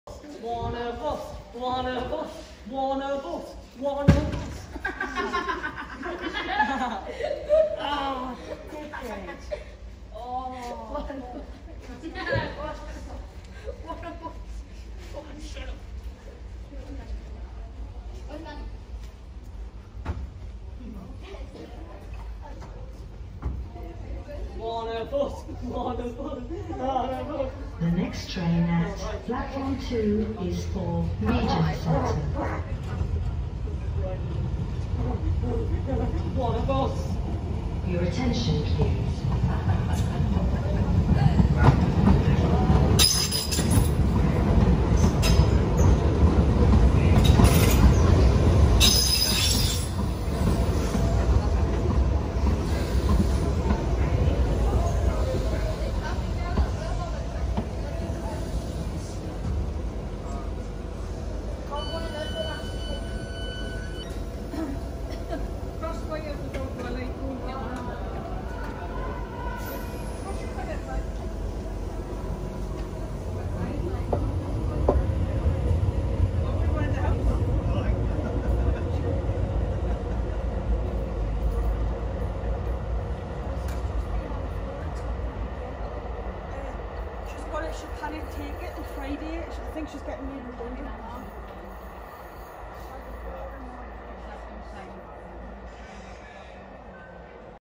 Metrocars 4037/4008 at Pelaw (14/07/2021)
In this video we see Metrocars 4037 and 4008 arriving into platform 2 at Pelaw for a short/peak service to Regent Centre